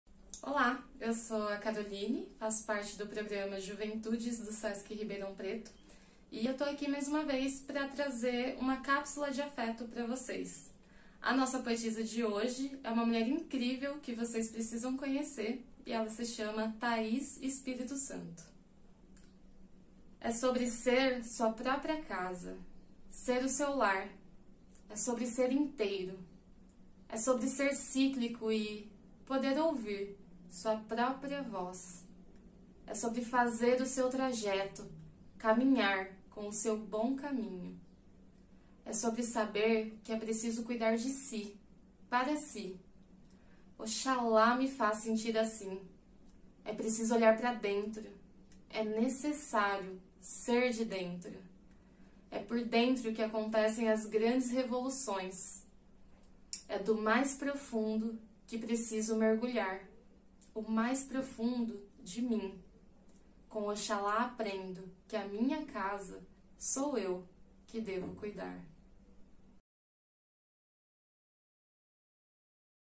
Juventudes - Cápsulas de Afeto
Cápsulas de Afeto apresenta a poetisa Taís Espírito Santo.